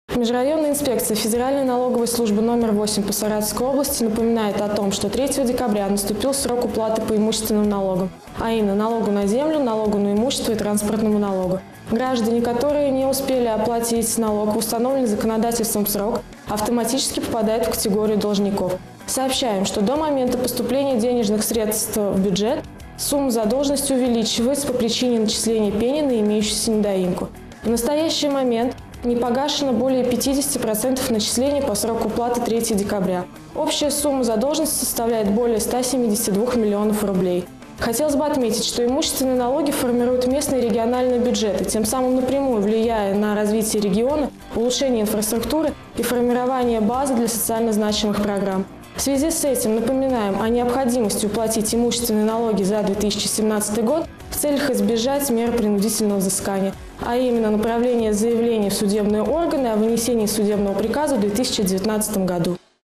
В новый год без долгов!
Объявление для граждан от налоговой инспекции.